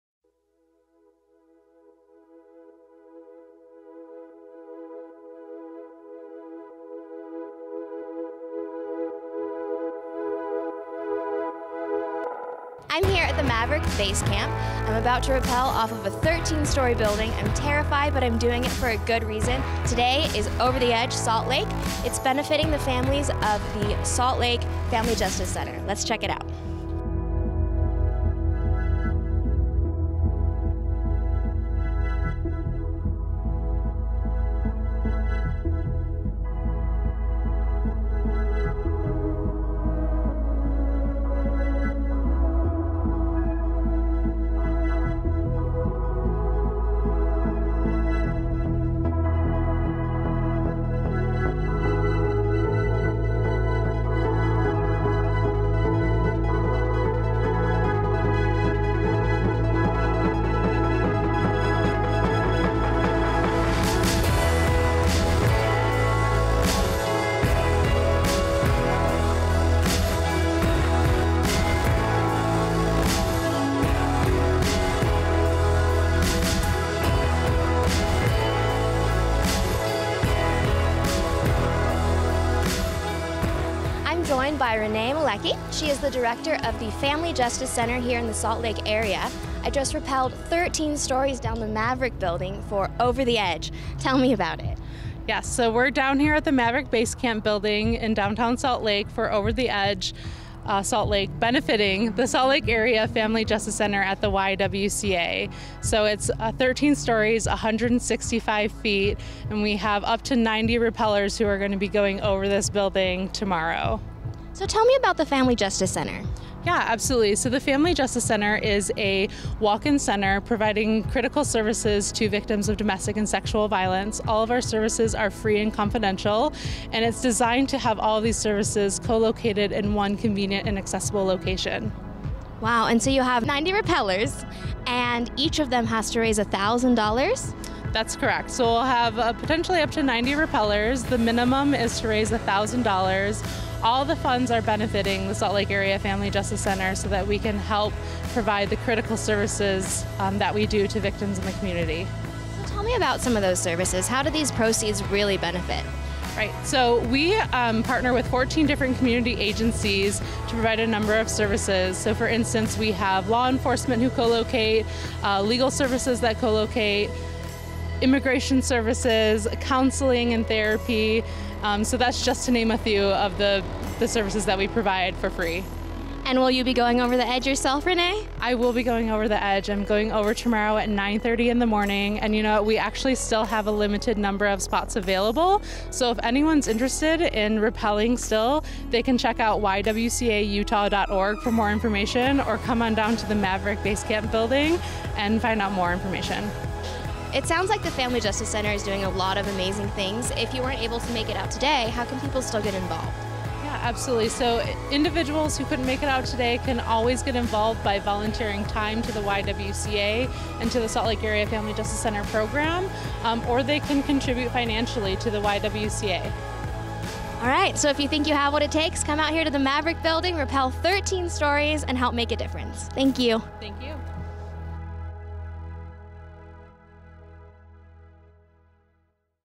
Over The Edge!
For Over The Edge Salt Lake, participants rappelled thirteen stories down the Maverik Base Camp Building in Downtown SLC to benefit Salt Lake Area Family Justice Center at the YWCA Utah